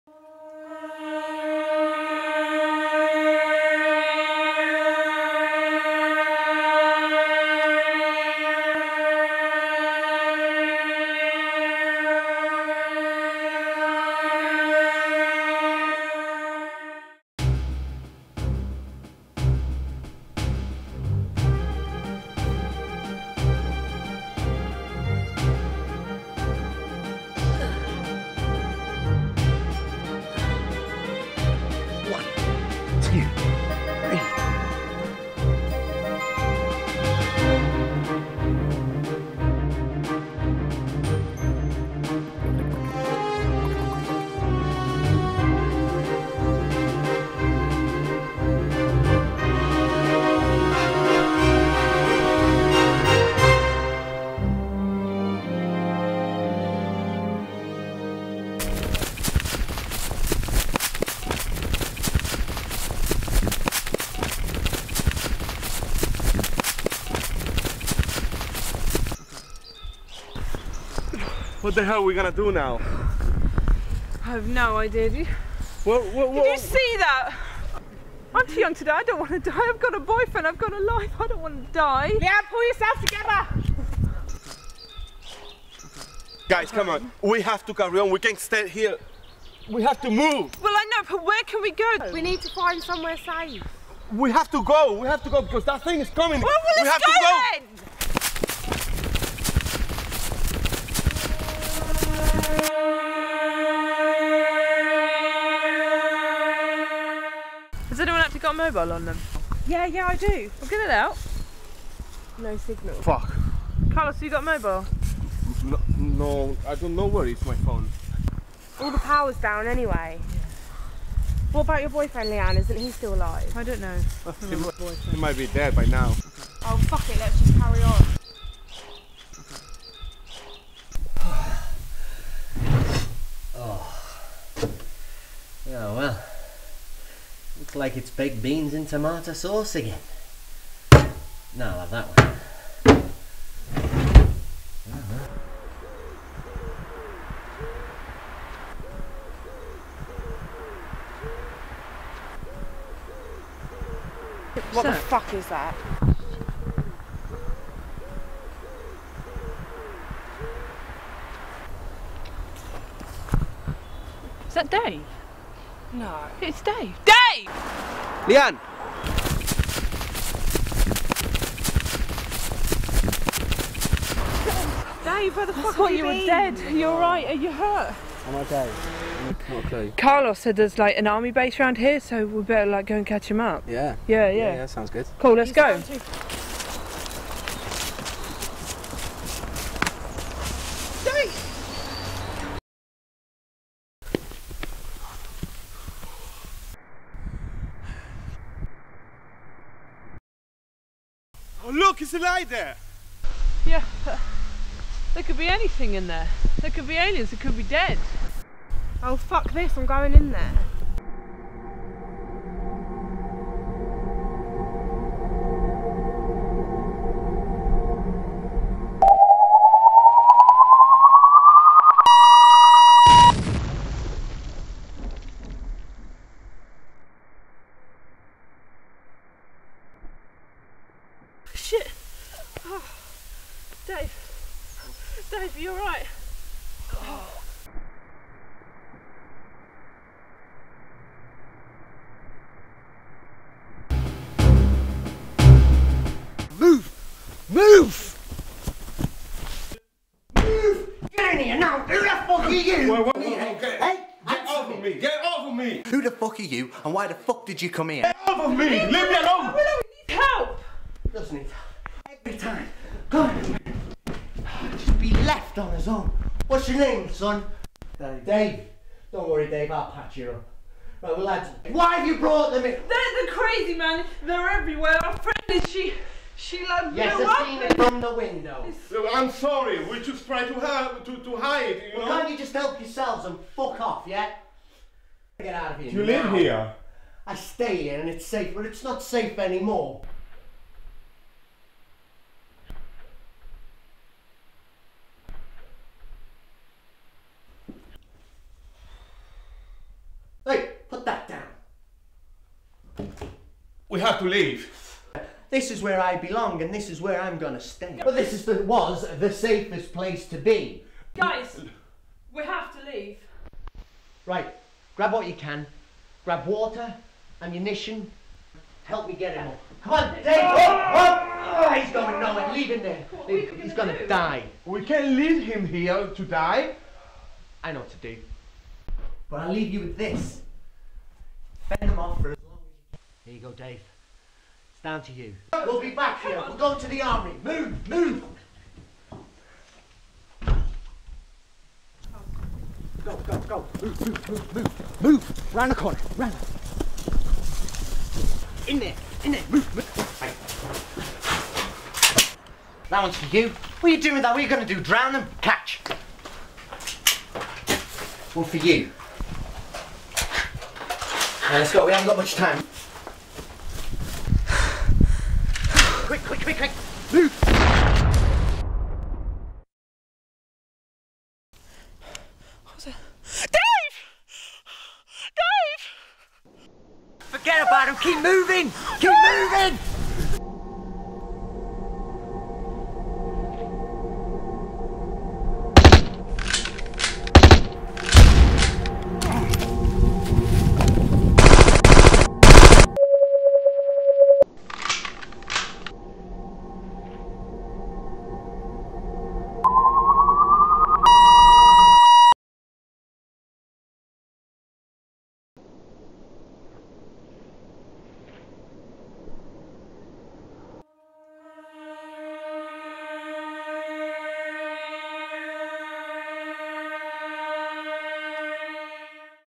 Day 7
Alien invasion short film, part of a student assignment
Using Premiere Pro, after effects, photoshop and a Canon 550D